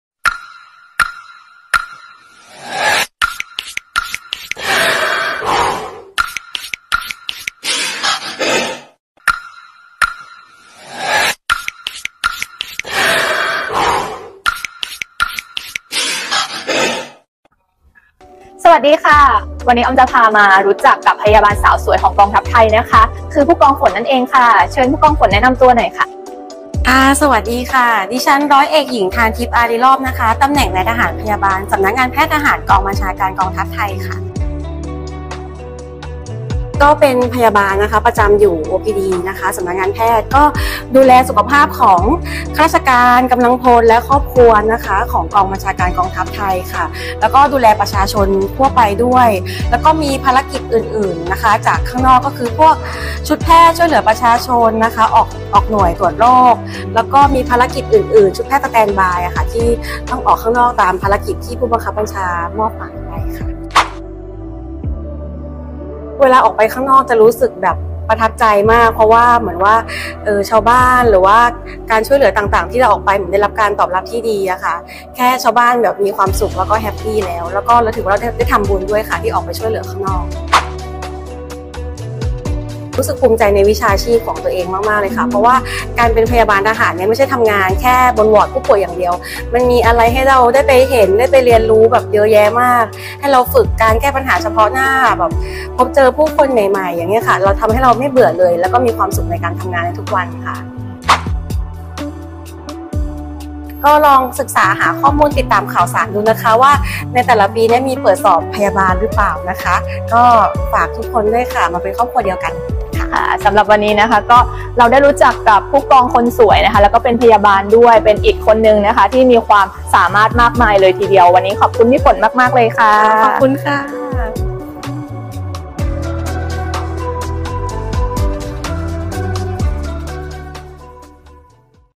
“คนสำคัญของกองทัพไทย” ตอน พยาบาลทหาร
มาทำความรู้จักกับ ร้อยเอกหญิง ธารทิพย์  อารีรอบ นายทหารพยาบาล สำนักงานแพทย์ทหาร กองบัญชาการกองทัพไทย ที่มีความภาคภูมิใจในวิชาชีพของตนเองที่ได้ใช้ในการช่วยเหลือกำลังพล ครอบครัว และประชาชน ในการให้บริการทางการแพทย์